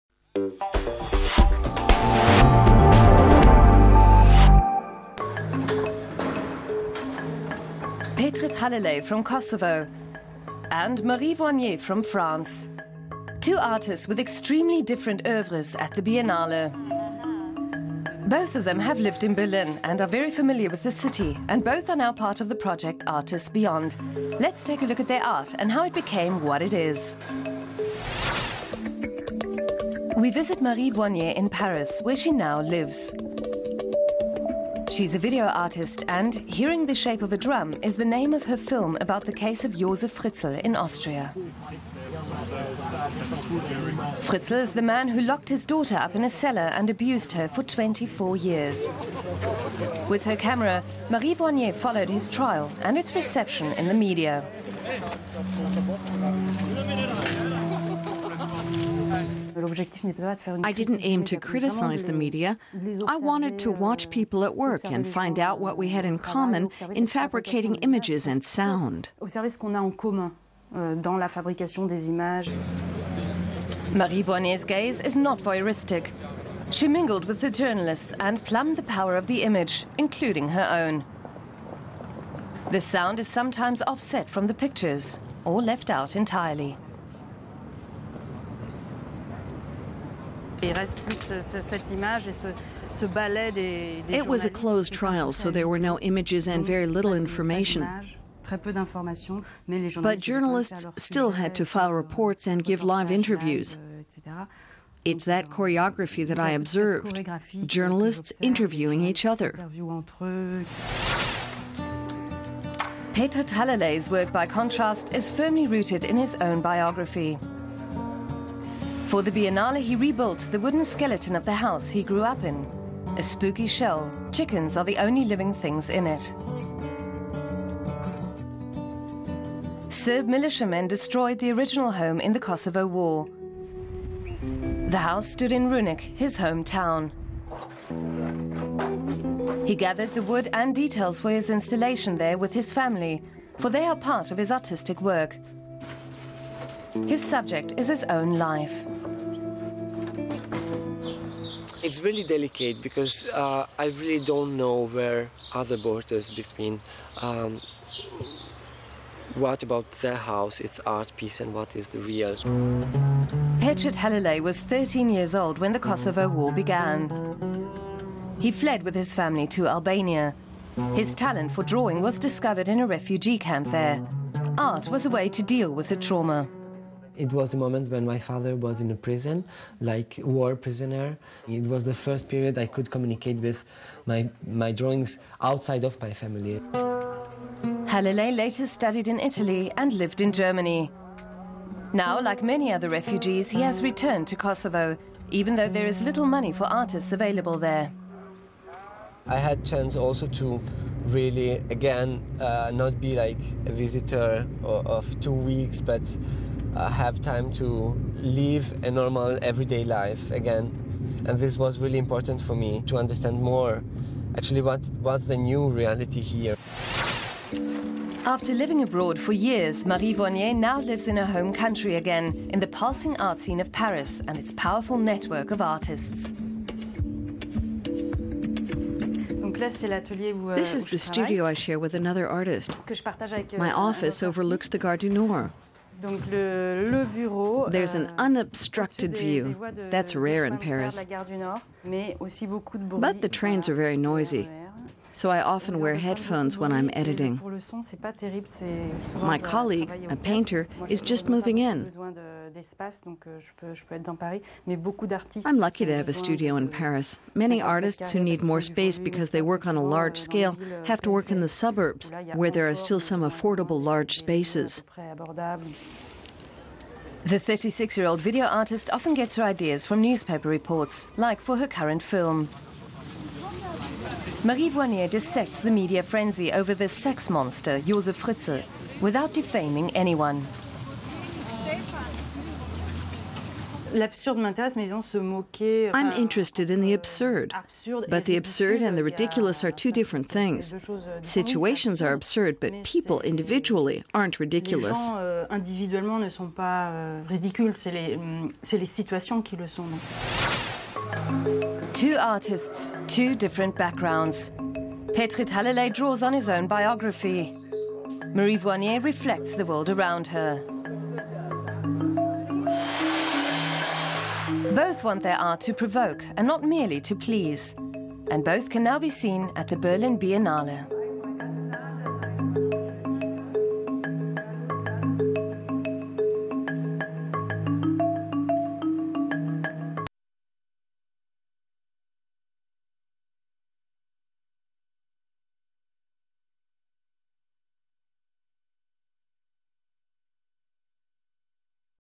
Where Art from Eastern and Western Europe Meet | Arts.21
We visit two Biennale artists at home: Petrit Halilaj from Pristina and Marie Voignier from Paris. Like many artists, they divide their time between their home countries and Berlin.